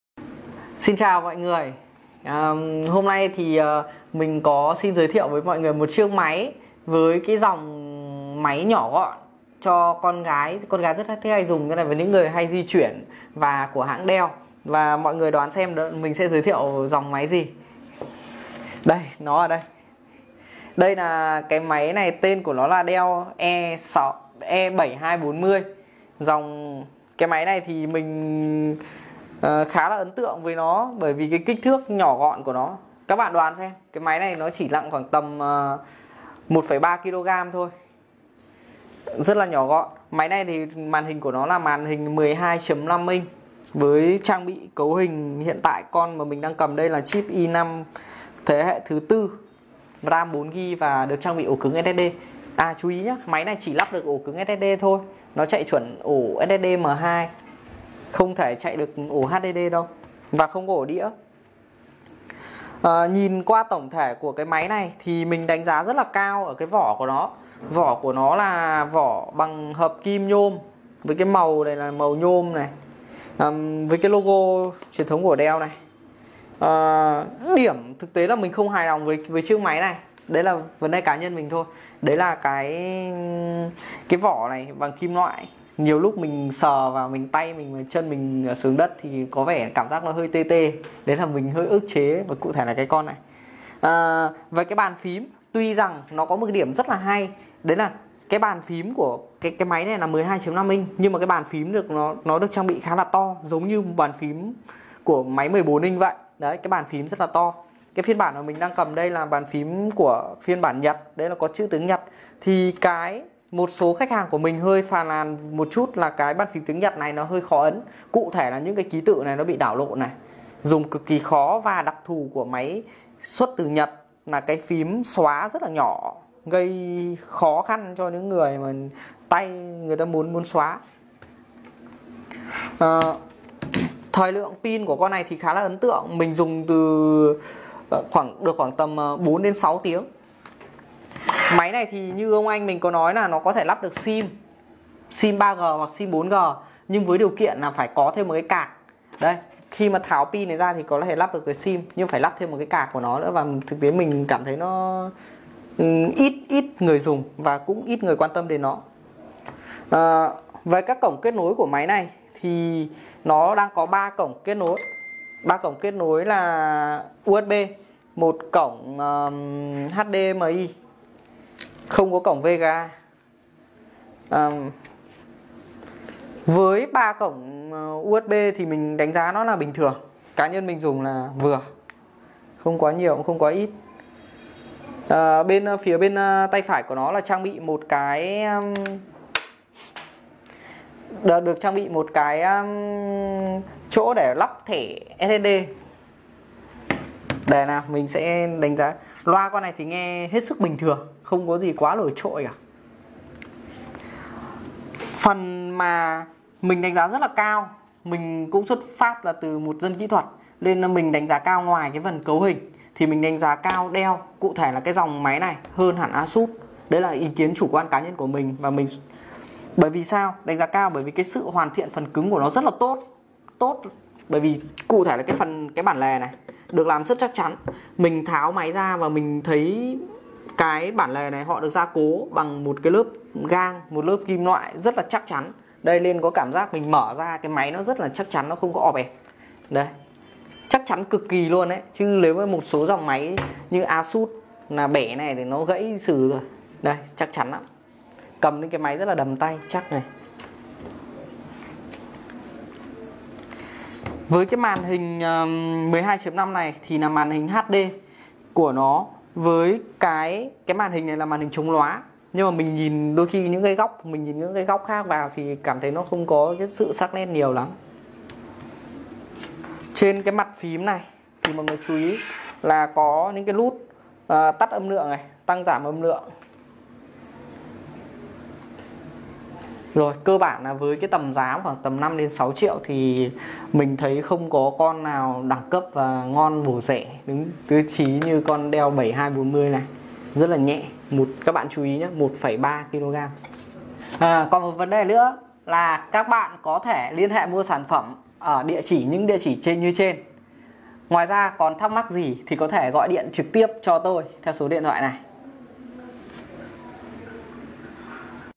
Laptop Dell nào mà Bền-Mỏng-Đẹp phù hợp với sinh viên?
với chiếc laptop dell latitude 7440 chắc chắn sẽ làm các bạn hài lòng
Cấu hình: chip corei5-4300U | Ram 4gb or 8gb | SSD 128gb or 256 gb | Card nvidia 4000 sẽ cho bạn chiến mọi loại game (trừ PUBG) nhé.